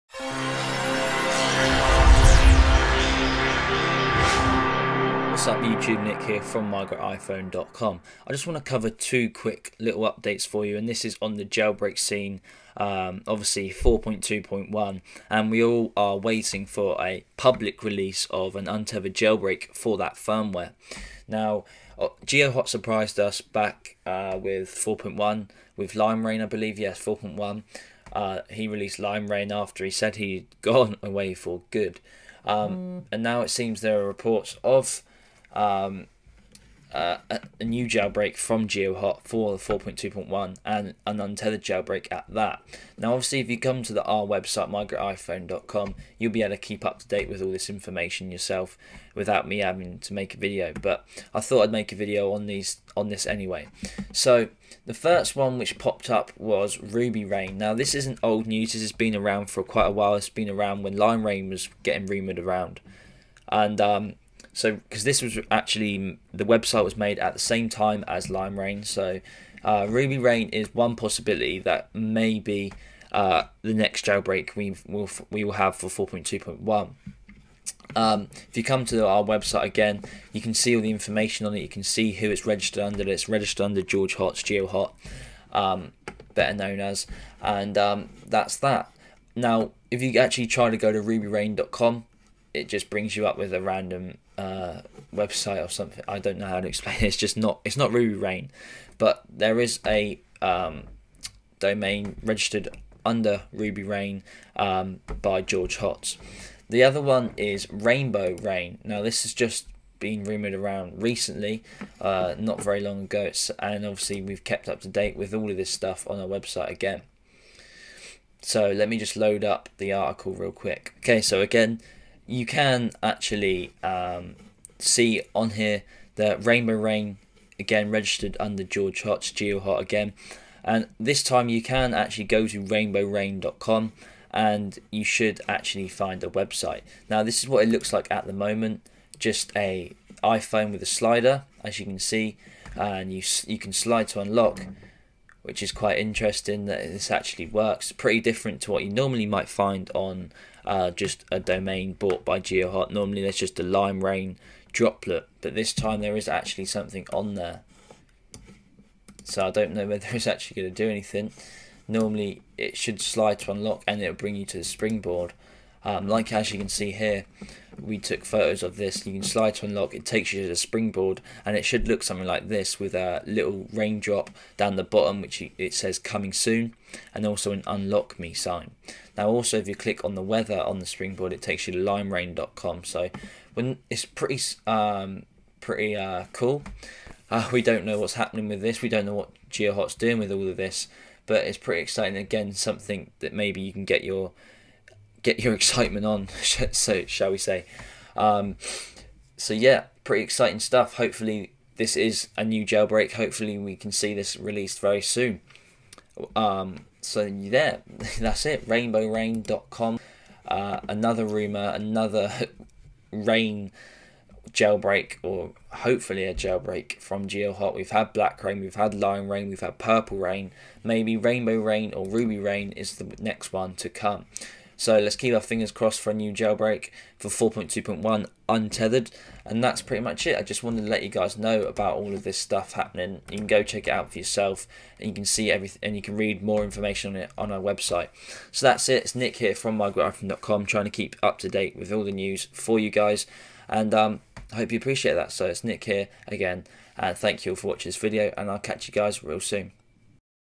RubyRa1n And Ra1nB0wRa1n - Next Jailbreaks From GeoHot?
This is a quick simple video to inform you guys of some very interesting and exciting rumors. With Geohotz surprising return earlier this year with his release of Limera1n, it wouldn't surprise us again if he was to come out with an untethered jailbreak for iOS 4.2.1. The rumors going around are that its going to be called RubyRa1n or possibly Ra1nB0wRa1n. What ever the name, its all very exciting and hopefully this rumor will come true very soon.